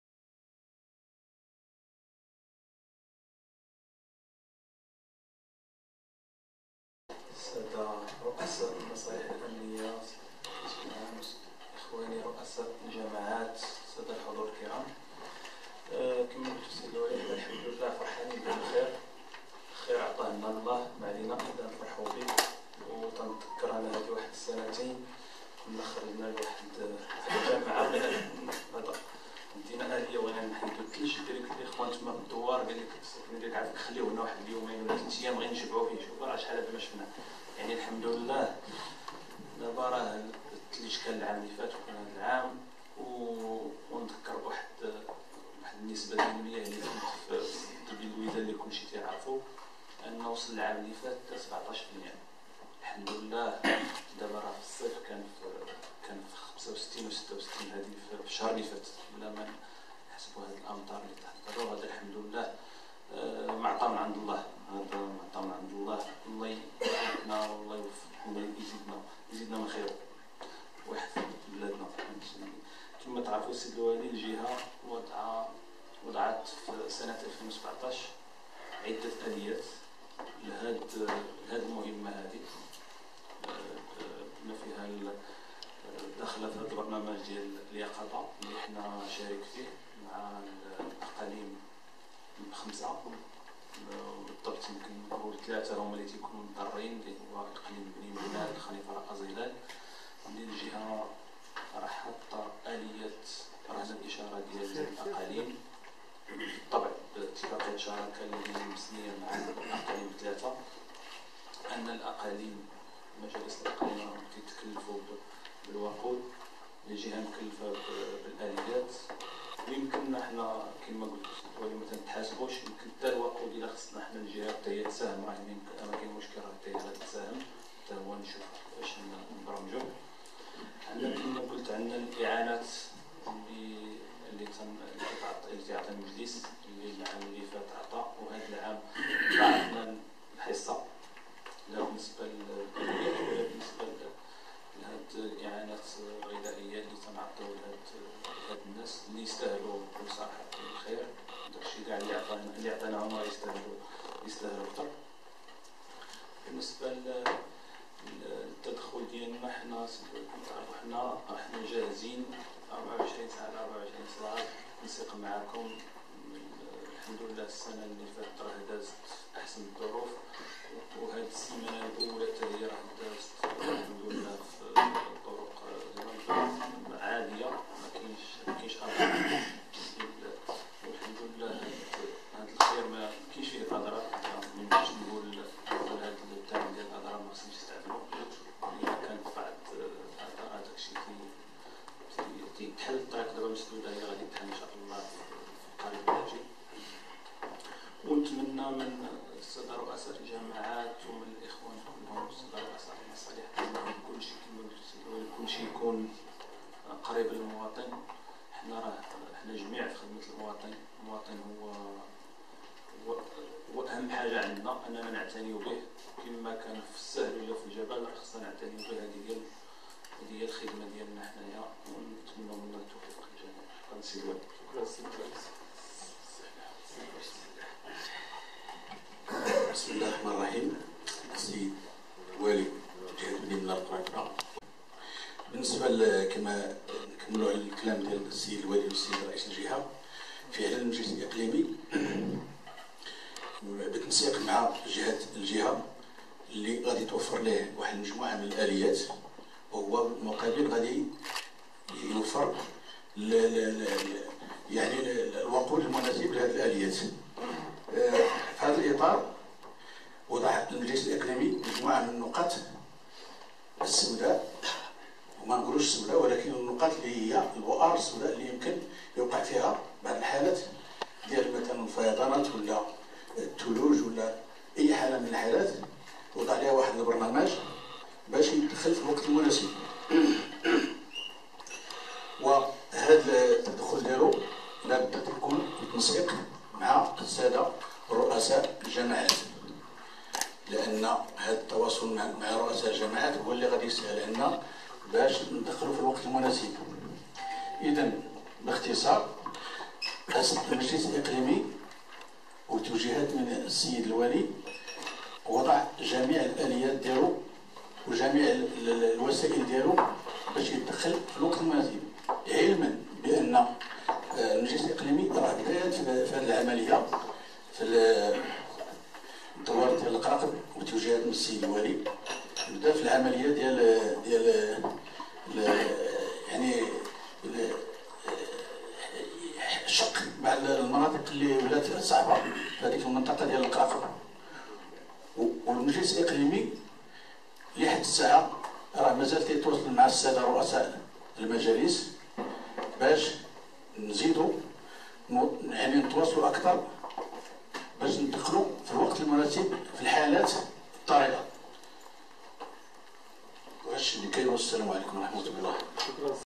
مداخلة مجاهد وحلحال خلال لقاء اتخاذ الإجراءات اللازمة "لمواجهة موجة البرد" بإقليم بني ملال
مداخلتي كل من إبراهيم مجاهد رئيس مجلس جهة بني ملال خنيفرة، ومحمد حلحال رئيس المجلس الإقليمي لبني ملال، خلال الاجتماع الذي عقد بمقر ولاية جهة بني ملال خنيفرة برئاسة السيد عبد السلام بكرات والي جهة بني ملال خنيفرة وعامل إقليم بني ملال صباح اليوم الخميس 1 نونبر 2018 لمدارسة الاجراءات الاستباقية لمواجهة موجة البرد بالإقليم